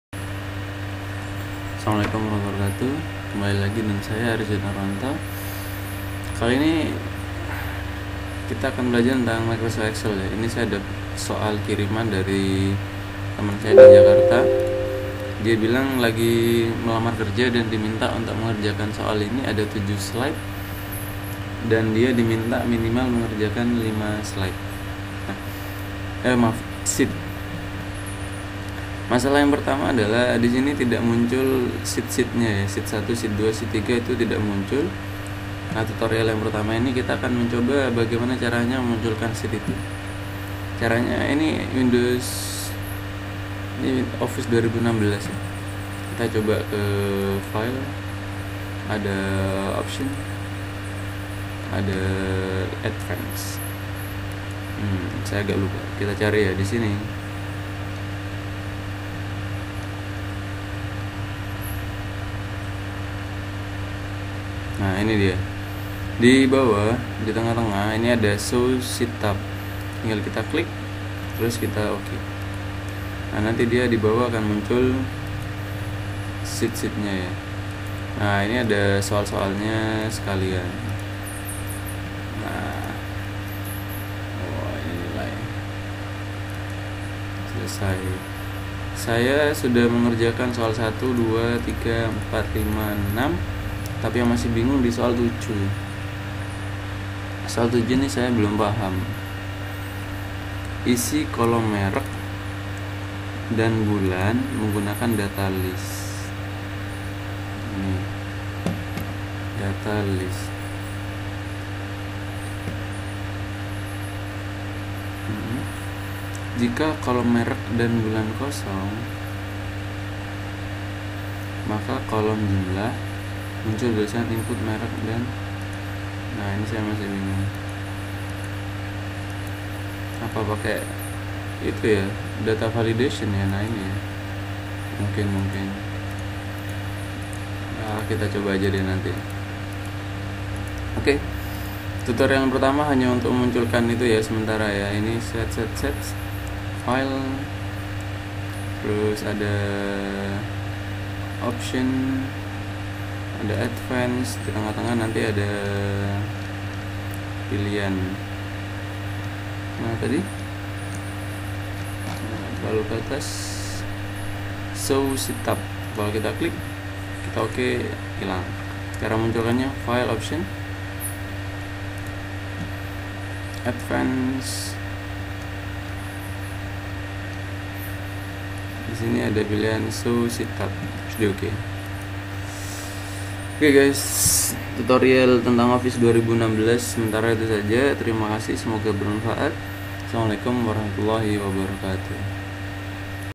Cara Memunculkan dan Menyembunyikan Sheet di Microsoft Excel 2016
Salah satu komponen utama dari tampilan microsoft excel 2016 adalah sheet. Itu yang di bawah, ada sheet1, sheet2 dan sheet3, kalau sheet ini gak muncul, biasanya ya kita bingung mau pergi ke sheet berikutnya. Kadang hal ini disembunyikan untuk hal-hal tertenut. Untuk memunculkannya. temen-temen bisa mengikuti langkah-langkah pada video diatas.
1. klik file
2. pilih option
3. klik option
4. cawang pada pilihan 'show sheet tabs'
5. klik ok
6. selesai